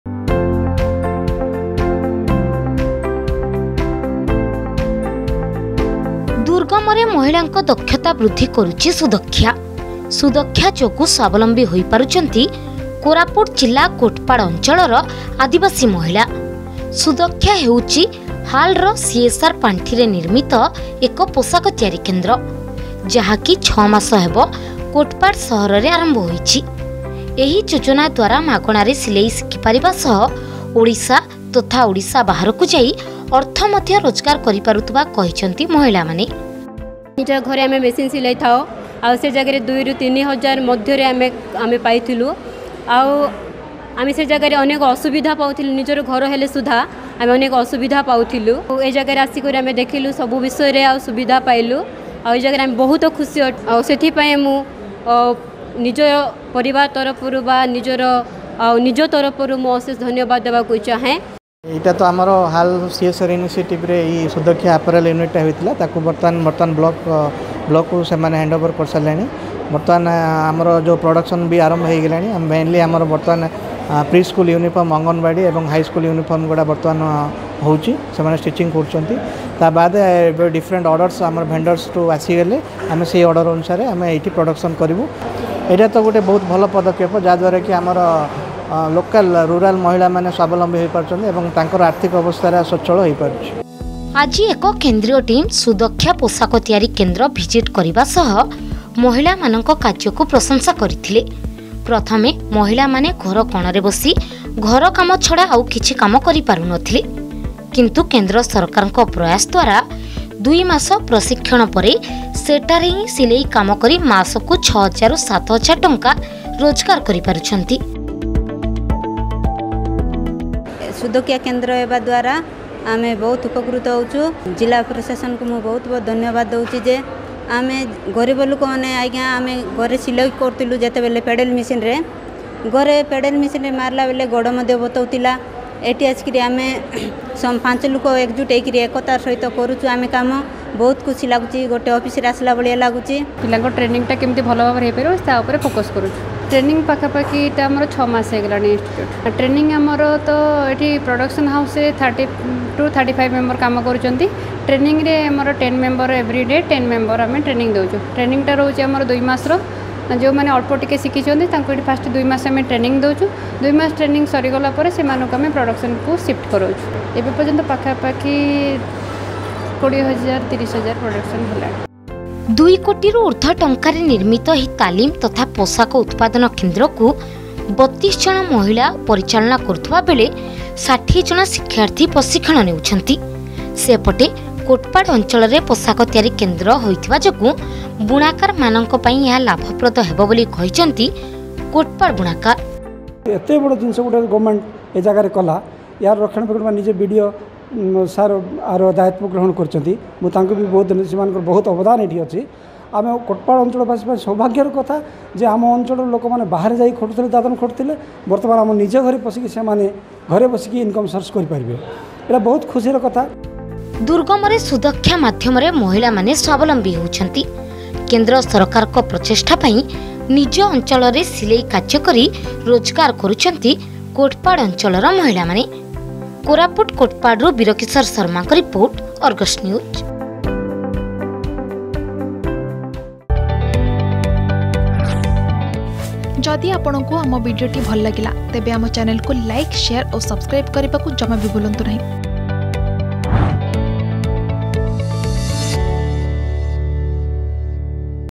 ମହିଳାଙ୍କ ଦକ୍ଷତା ବୃଦ୍ଧି କରୁଛି ସୁଦକ୍ଷା । ସୁଦକ୍ଷା ଯୋଗୁଁ ସ୍ବାବଲମ୍ବୀ ହୋଇପାରୁଛନ୍ତି କୋଟପାଡ଼ ଅଞ୍ଚଳର ମହିଳା ।
ଦୁର୍ଗମରେ ମହିଳାଙ୍କ ଦକ୍ଷତା ବୃଦ୍ଧି କରୁଛି ସୁଦକ୍ଷା । ସୁଦକ୍ଷା ଯୋଗୁଁ ସ୍ବାବଲମ୍ବୀ ହୋଇପାରୁଛନ୍ତି କୋରାପୁଟ ଜିଲ୍ଲା କୋଟପାଡ଼ ଅଞ୍ଚଳର ଆଦିବାସୀ ମହିଳା । ସୁଦକ୍ଷା ଯୋଗୁଁ ମାଗଣାରେ ସିଲେଇ ଶିଖି ରୋଜଗାର କରିପାରୁଛନ୍ତି ମହିଳା।